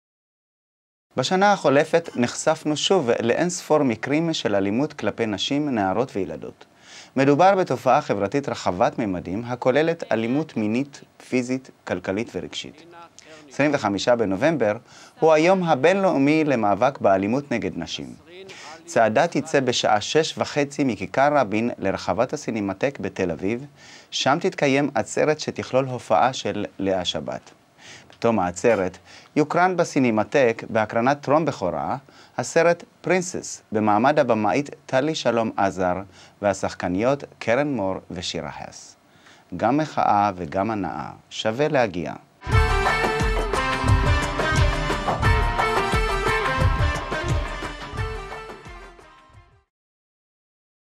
צעדת היום למאבק באלימות נגד נשים
בשנה החולפת נחשפנו שוב, לאין ספור מקרים של אלימות כלפי נשים, נערות וילדות. מדובר בתופעה חברתית רחבת ממדים, הכוללת אלימות מינית, פיזית, כלכלית ורגשית. 25 בנובמבר הוא היום הבינלאומי למאבק באלימות נגד נשים. צעדה תצא בשעה שש וחצי מכיכר רבין לרחבת הסינמטק בתל אביב, שם תתקיים עצרת שתכלול הופעה של לאה שבת. בתום העצרת, יוקרן בסינמטק, בהקרנת טרום בכורה, הסרט "פרינסס", במעמד הבמאית טלי שלום עזר והשחקניות קרן מור ושירה האס. גם מחאה וגם הנאה - שווה להגיע.